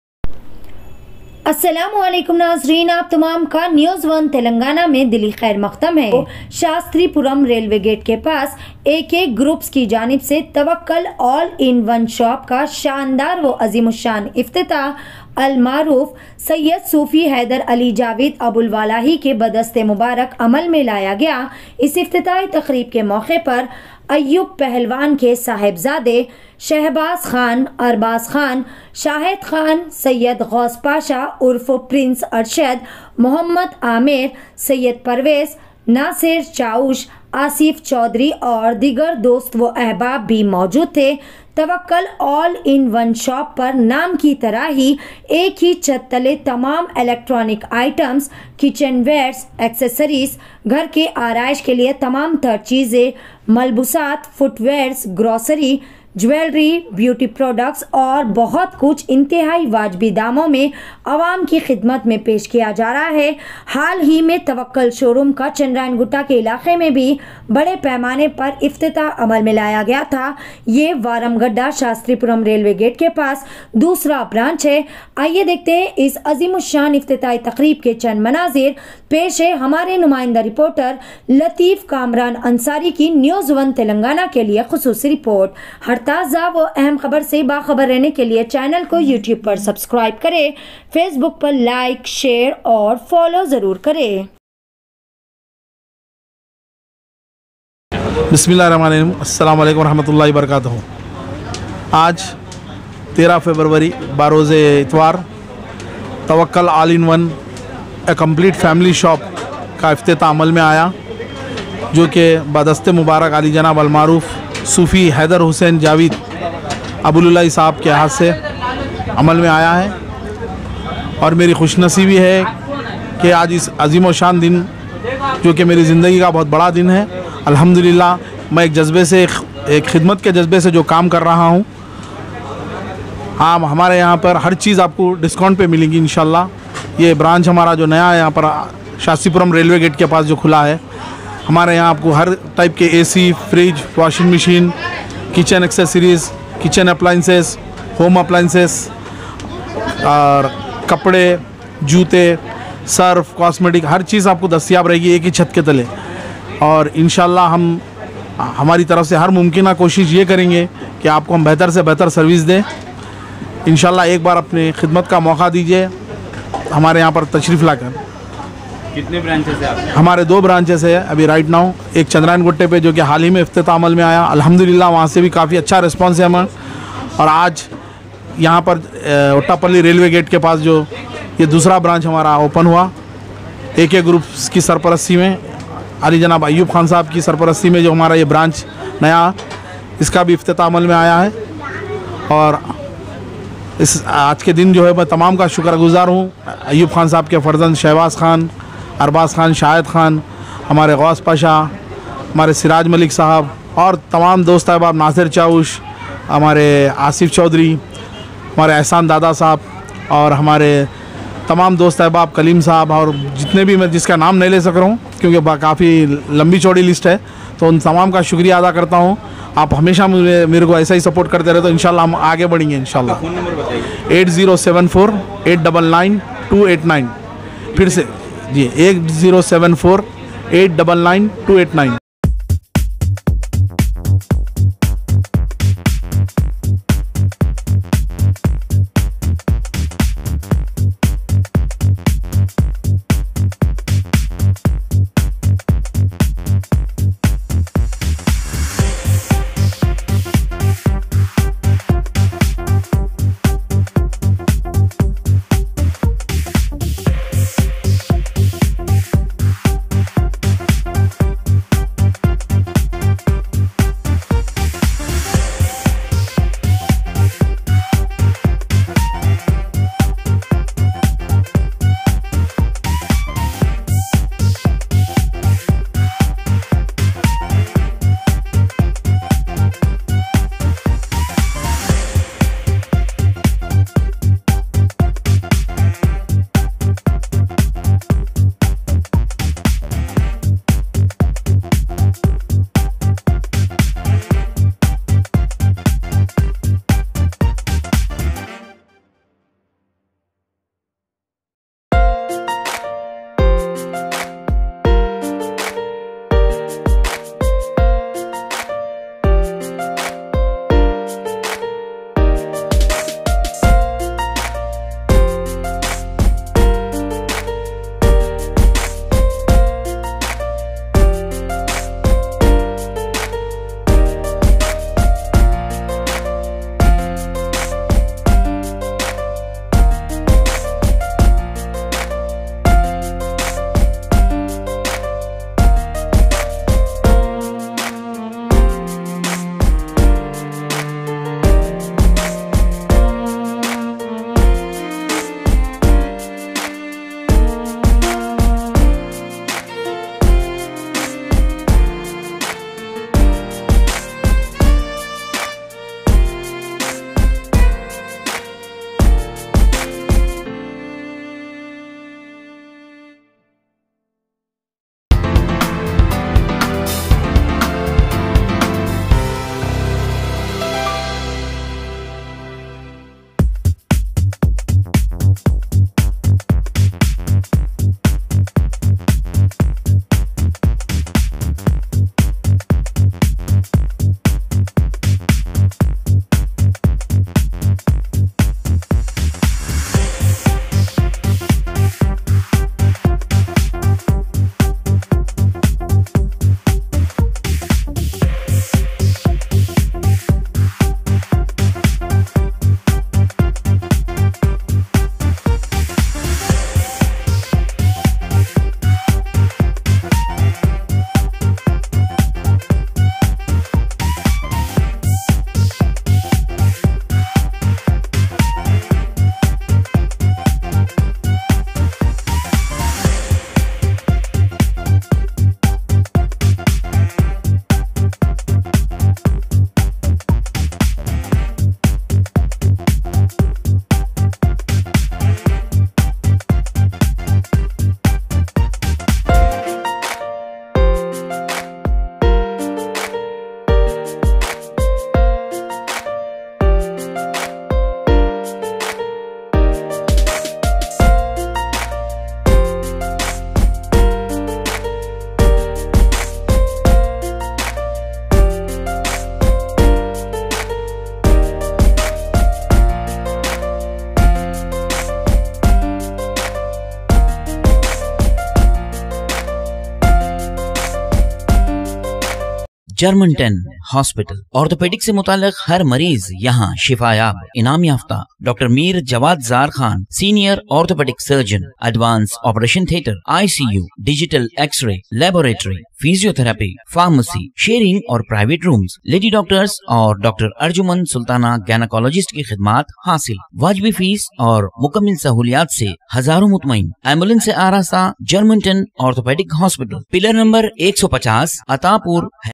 Ak Groups Tawakkal All in One Shop inauguration @ Shastripuram Railway Gate Hyderabad|| NEWS 1
Ak Groups Tawakkal All in One Shop inauguration @ Shastripuram Railway Gate Hyderabad|| NEWS 1 TELANGANA.